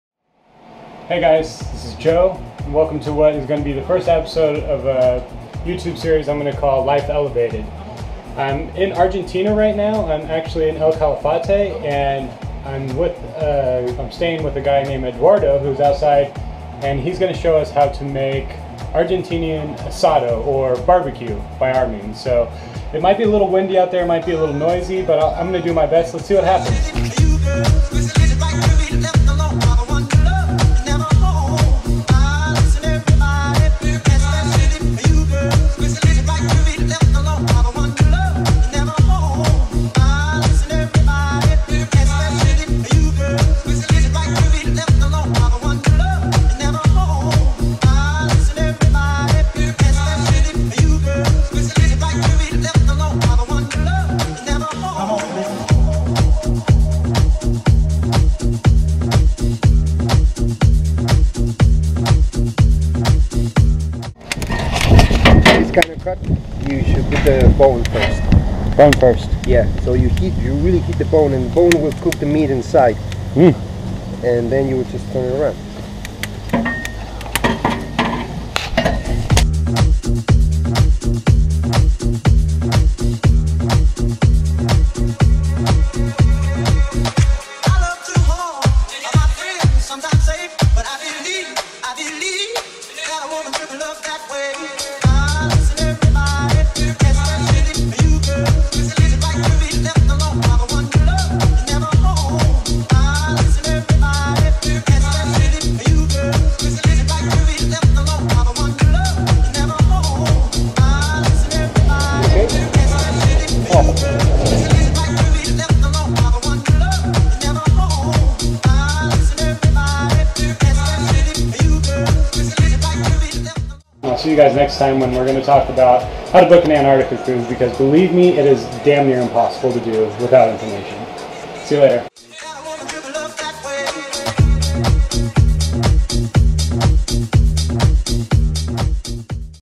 Life Elevated #1
Life Elevated Episode 1! Ventured down to El Calafate in Argentina and learned the secret to Argentinean BBQ! The secret is. go to Argentina!